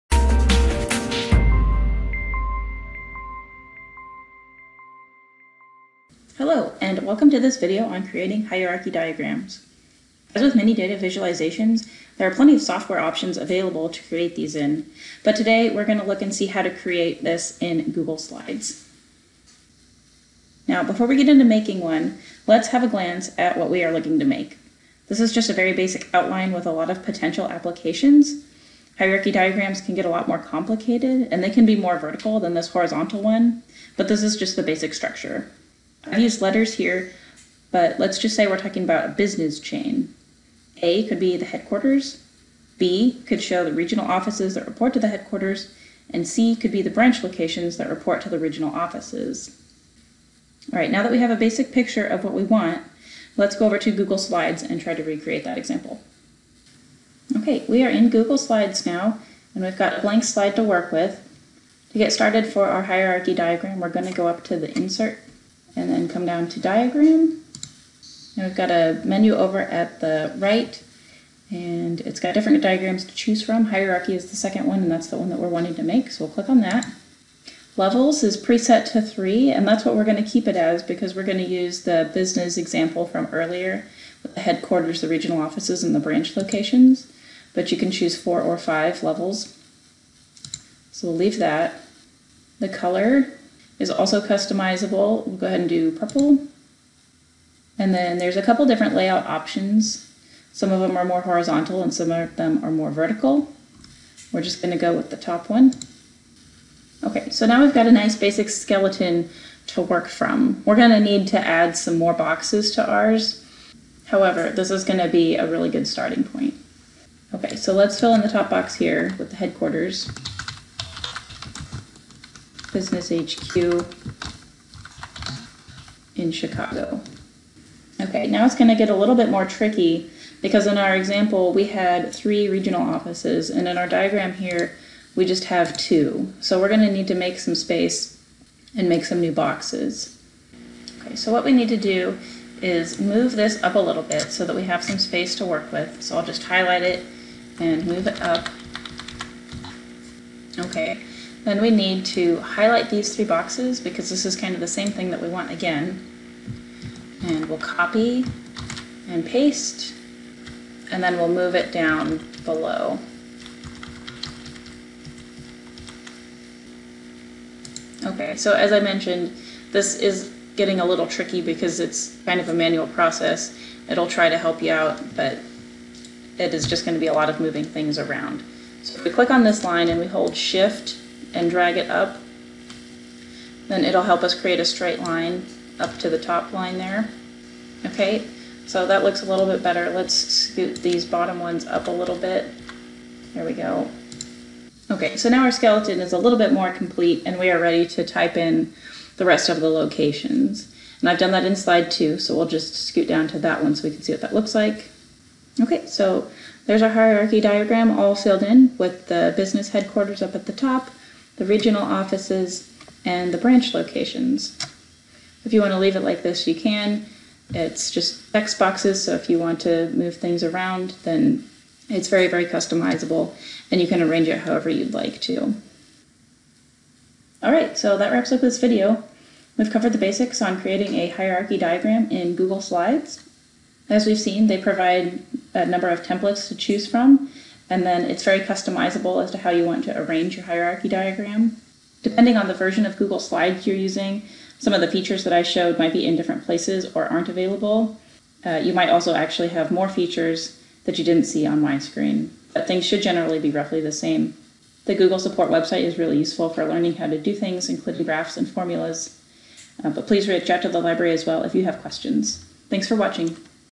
Creating Hierarchy Diagrams in Google Slides
This video covers the basics on creating a hierarchy diagram in Google Slides.